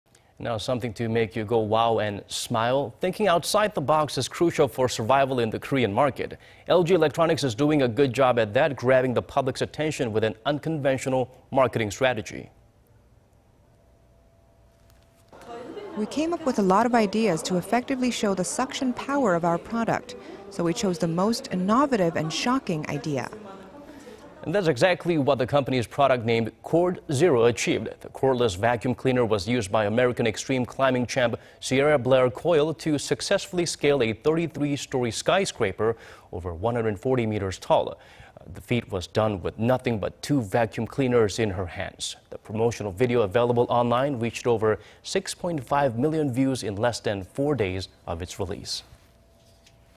World climbing champion climbs a 33-story building with LG's vacuum cleaner
이색 마케팅 눈길...LG 진공청소기 힘으로 33층 빌딩 올랐다

Thinking outside the box is crucial for survival in the Korean market.
LG Electronics is doing a good job at that... grabbing the public' attention with an unconventional marketing strategy.

"We came up with a lot of ideas to effectively show the suction power of our product. So we chose the most innovative and shocking idea."

And that's exactly what the company's product named Cord Zero achieved.
The cordless vacuum cleaner was used by American extreme climbing champ, Sierra Blair-Coyle, to successfully scale a 33-story skyscraper...over 140-meters tall.
The feat was done with nothing but two vacuum cleaners in Blair's hands.
The promotional video available online reached over 6-point-5 million views in less than four days of its release.